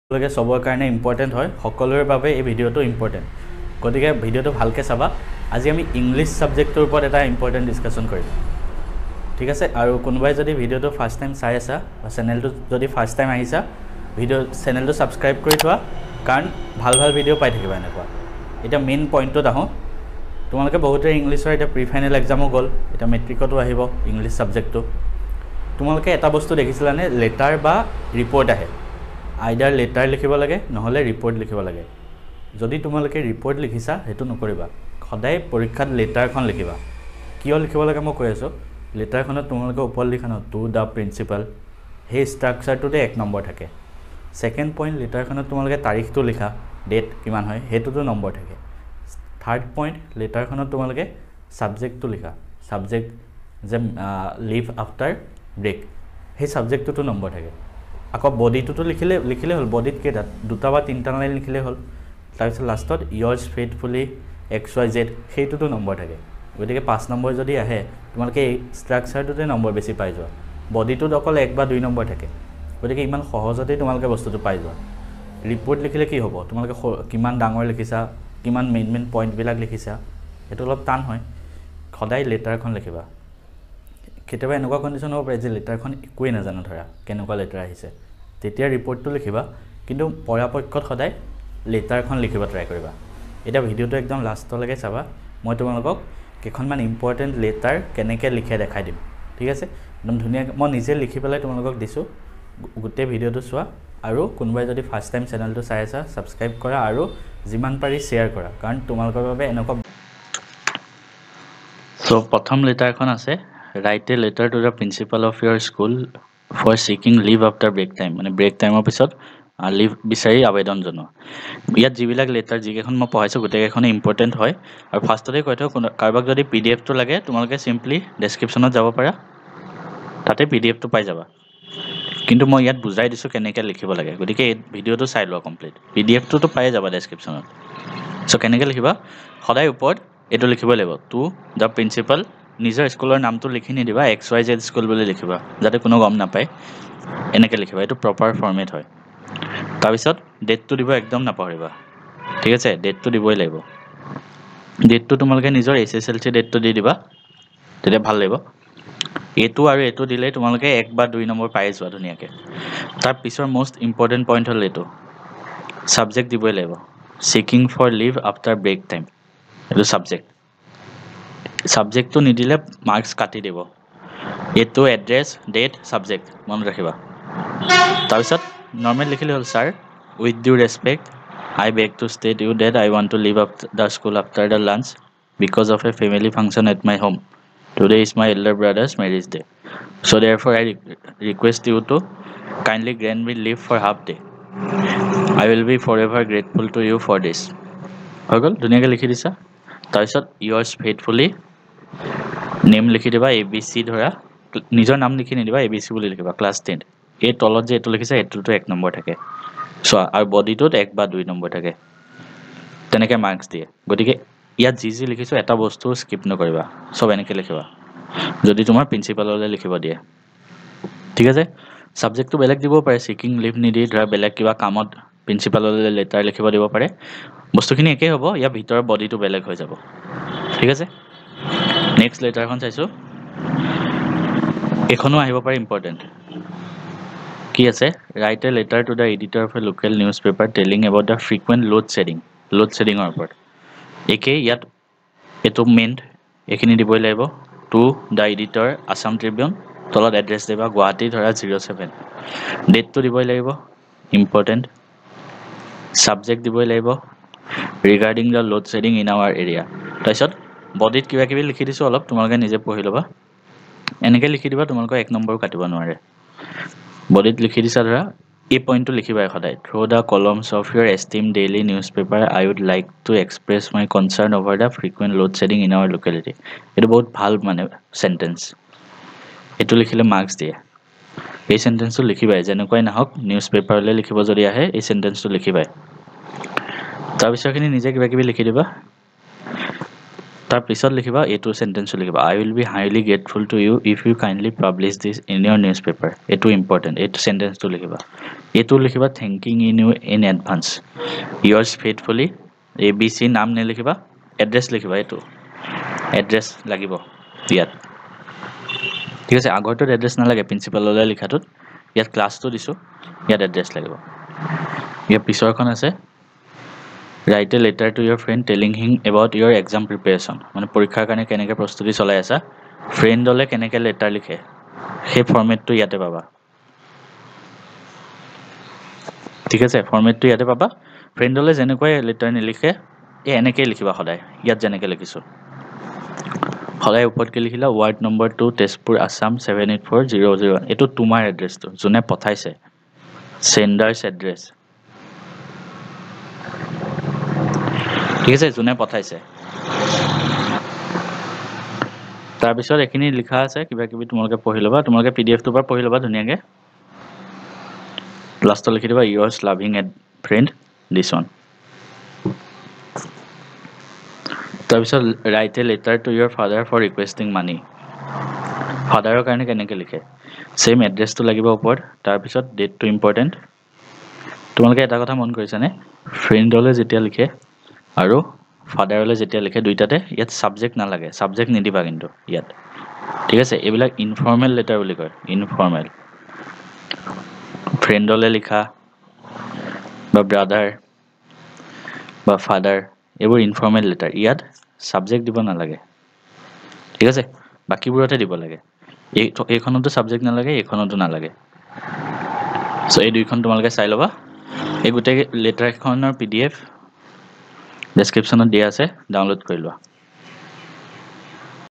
English Important letter for HSLC 2022 || Letter or Report? কোনটো লিখিলে বেছি নম্বৰ পায়
Advance maths important question